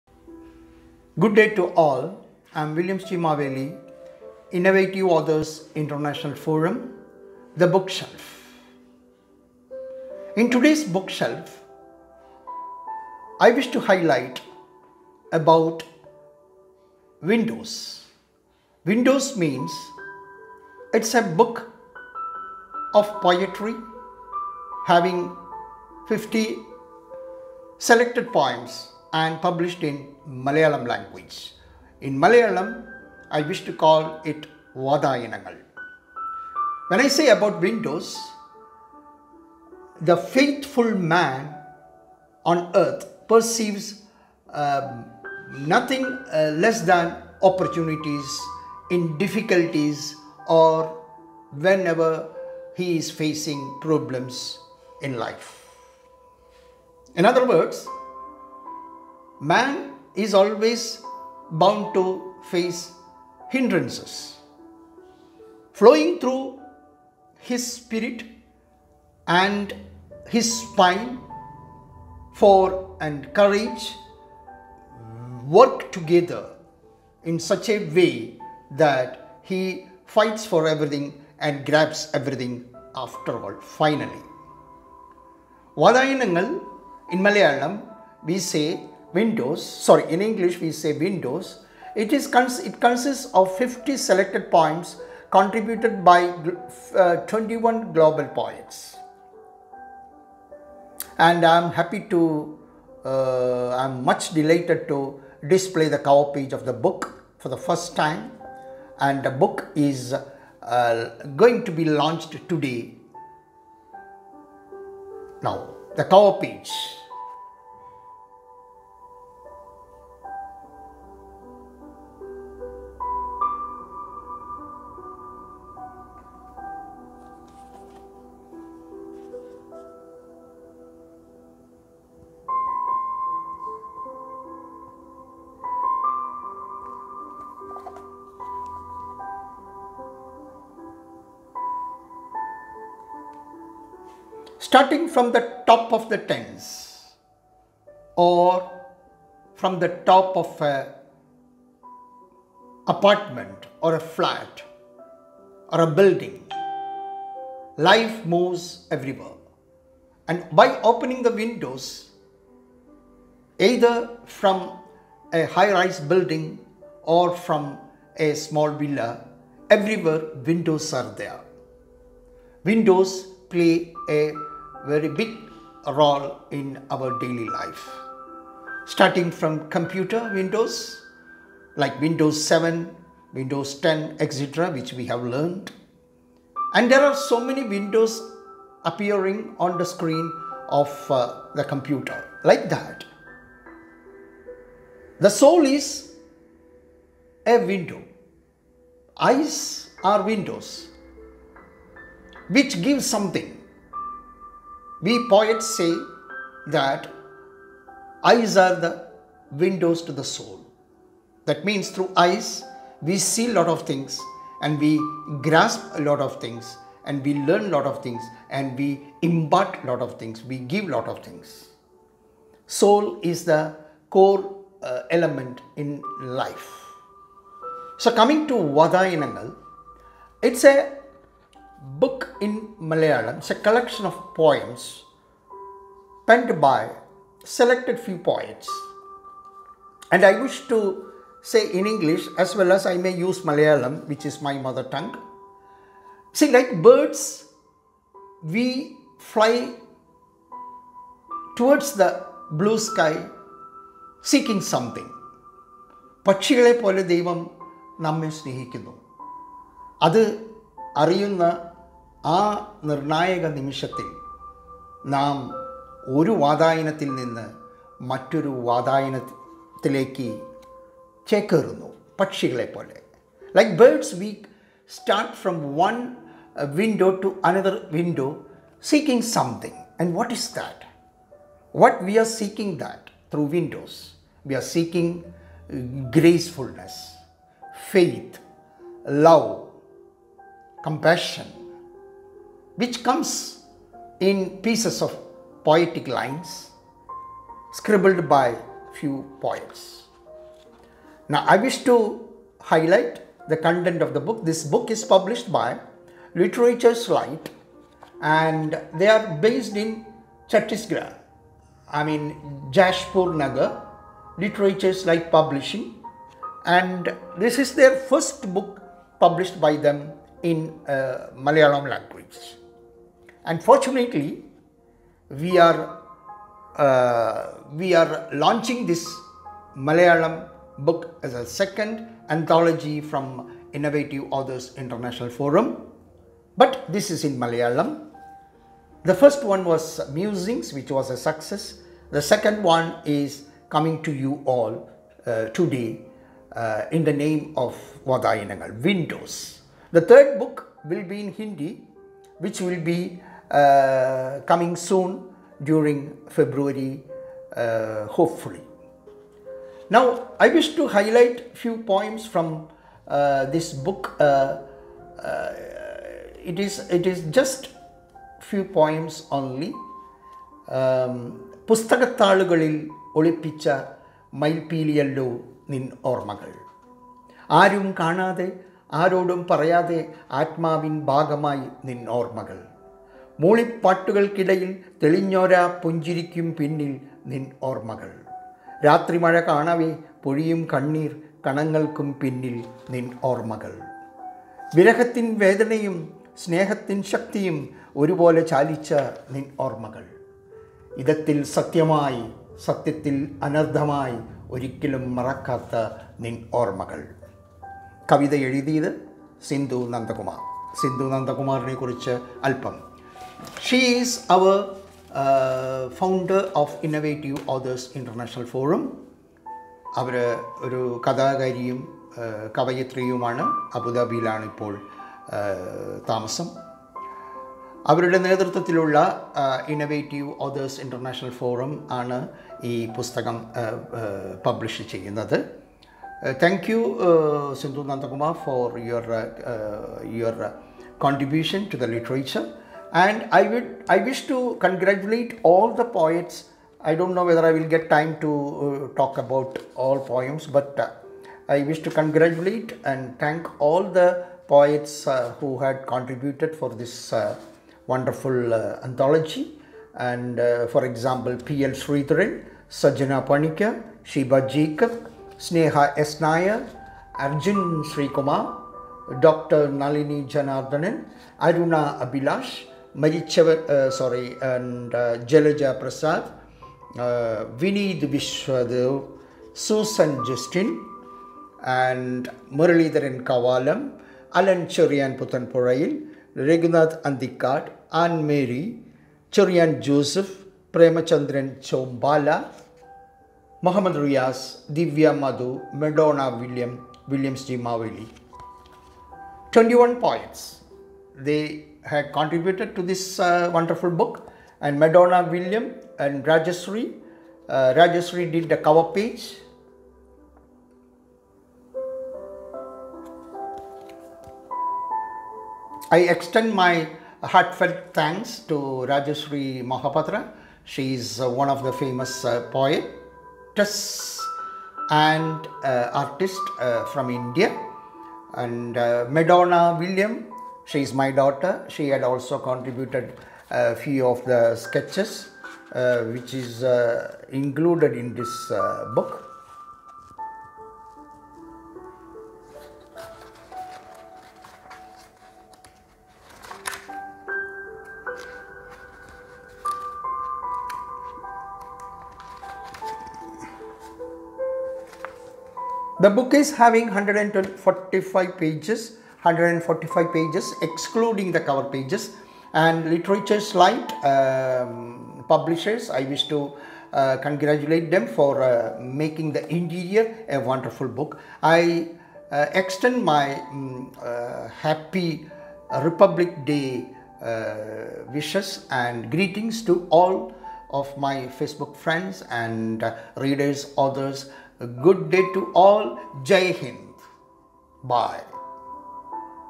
The bookshelf
Book on review: Vadayanangal
Reviewed by : Williamsji Maveli
Creative director